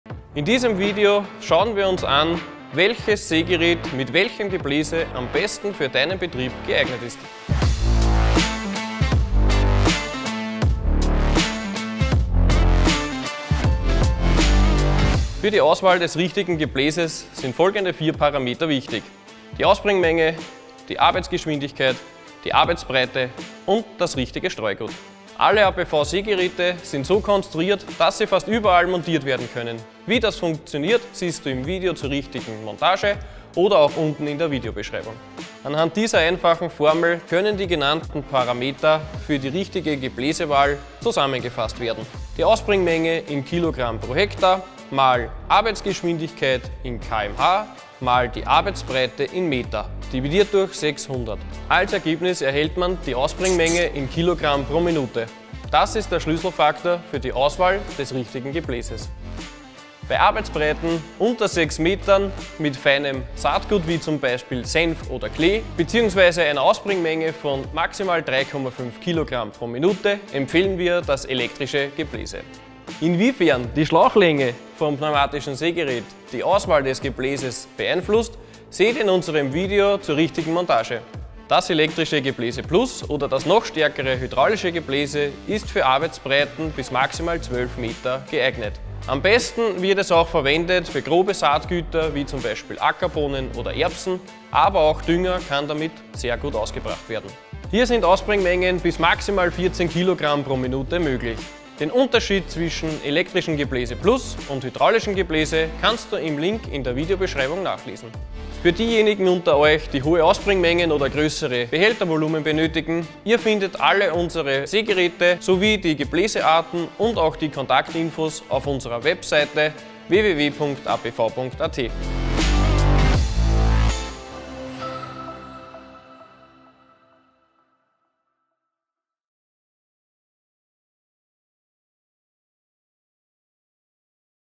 Sägerät + Gebläseart: Welche Auswahl passt zu deinem Betrieb?
EN-SUBTITLES AVAILABLE
In diesem Video erklären wir, nach welchen Kriterien die Auswahl des idealen Sägeräts für deinen Betrieb erfolgt und mit welcher Gebläseart du am effektivsten arbeitest.

Transkript:
In diesem Video schauen wir uns an, welches APV Sägerät mit welchem Gebläse am besten für deinen Betrieb geeignet ist.

Für die Auswahl des richtigen Gebläses sind folgende 4 Paremeter wichtig:
- Ausbringmenge
- Arbeitsgeschwindigkeit
- Arbeitsbreite
- Art des Streuguts.

Alle APV Sägeräte sind so konstruiert, dass sie fast überall montiert werden können. Wie das funktioniert siehst du im Video
zur richtigen Montage oder auch in der Videobeschreibung.

Anhand dieser einfachen Formel können die genannten Parameter für die richtige Gebläsewahl zusammengefasst werden:
(Ausbringmenge (kg/ha) x Arbeitsgeschwindigkeit (km/h) x Arbeitsbreite): 600 = Ausbringmenge (kg/min)

Das ist der Schlüssefaktor zur Auswahl des richtigen Gebläses. Bei Arbeitsbreiten unter 6 Metern mit feinem Saatgut wie z. B. Senf oder Klee bzw. einer Ausbringmenge von max. 3,5 kg/min empfehlen wir das elektrische Gebläse.

Inwiefern die Schlauchlänge vom pneumatischen Sägerät die Auswahl des Gebläses beeinflusst seht ihr in unserem Video zur richtigen Montage.

Das elektrische Gebläse PLUS oder das noch stärkere hydraulische Gebläse ist für Arbeitsbreiten bis max. 12 m geeignet.
Am besten wird es auch verwendet für grobes Saatgut wie z. B. Ackerbohnen oder Erbsen aber auch Dünger kann damit sehr gut ausgebracht werden. Hier sind Ausbringmengen bis max. 14 kg/min möglich.
Den Unterschied zwischen elektrischem Gebläse PLUS und hydraulischem Gebläse kannst du im Link in der Videobeschreibung nachlesen.

Für diejenigen unter euch, die hohe Ausbringmengen oder größere Behältervolumen benötigen, ihr findet alle unsere Sägeräte sowie die Gebläsearten und auch die Kontaktinfos auf unserer Webseite